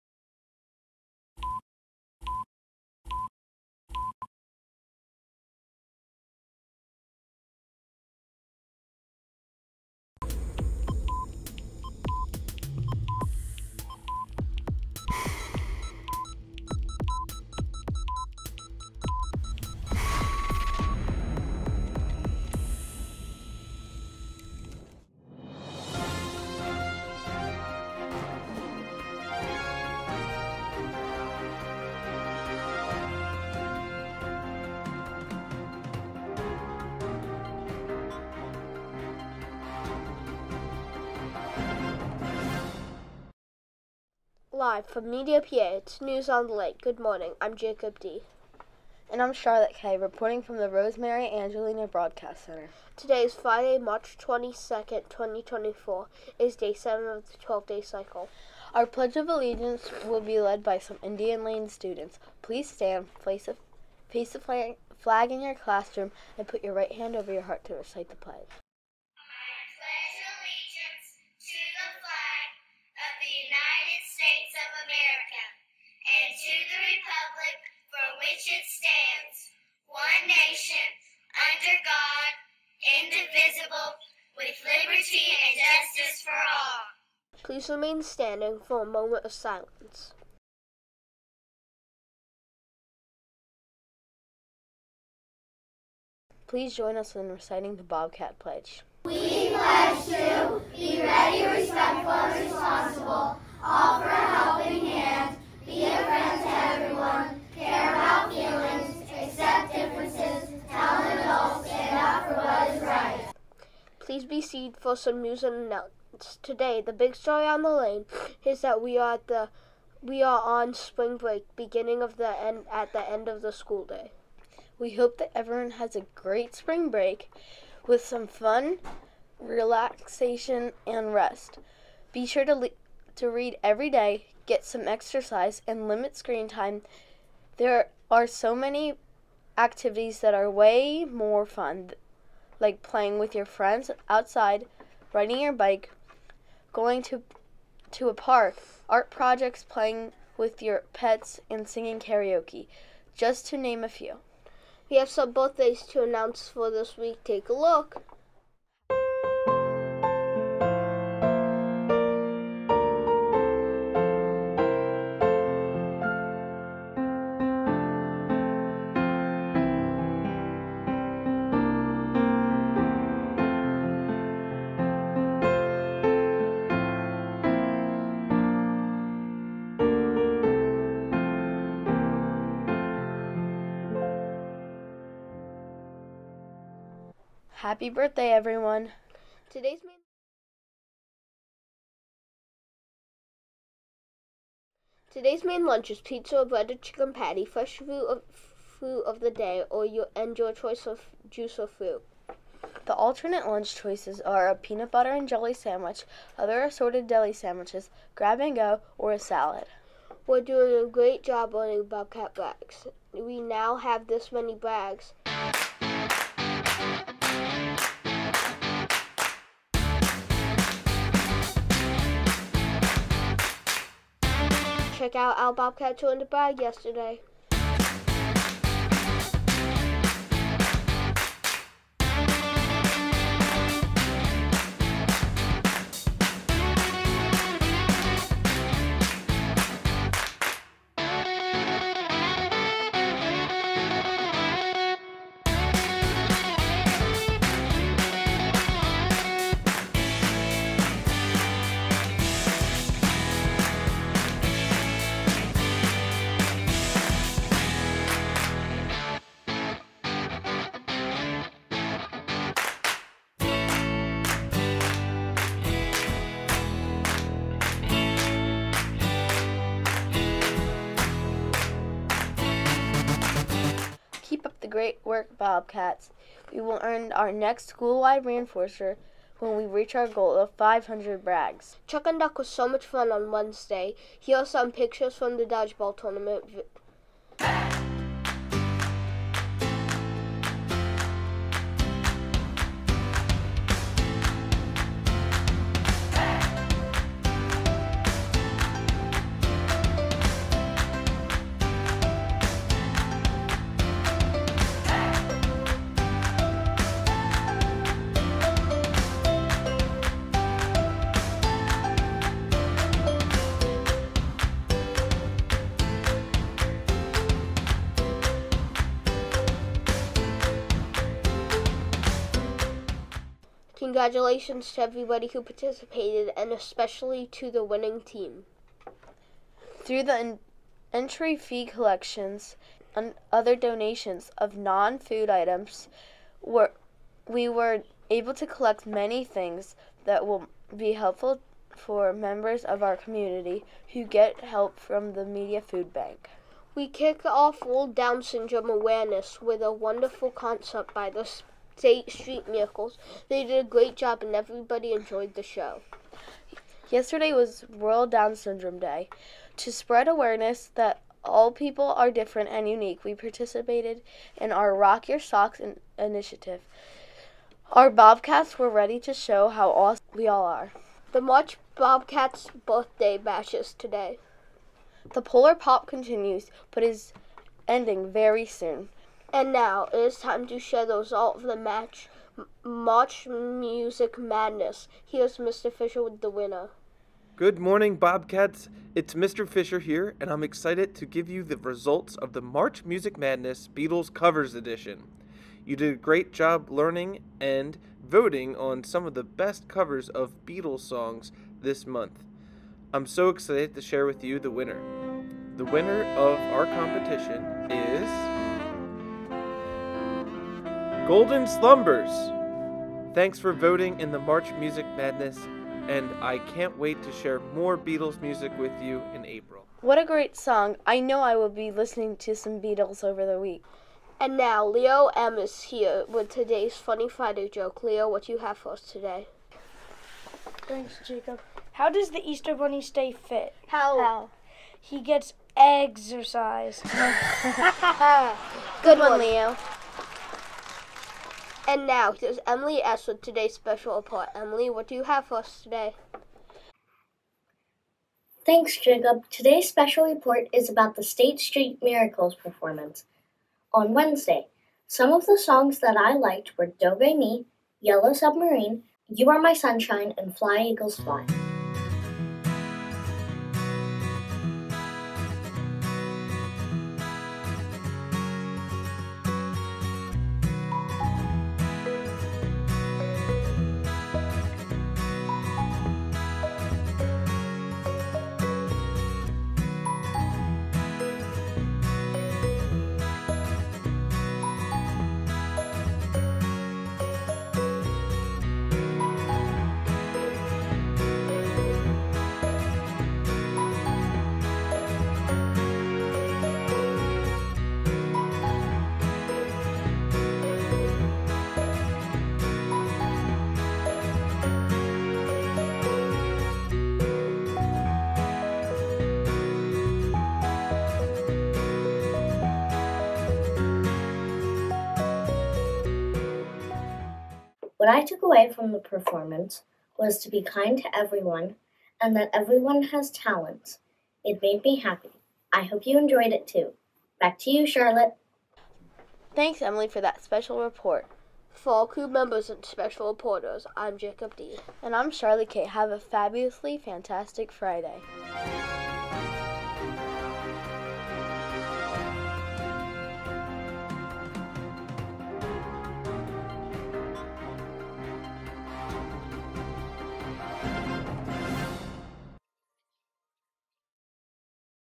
NOTL 3 22 24
Indian Lane's morning announcements for Friday, March 22, 2024.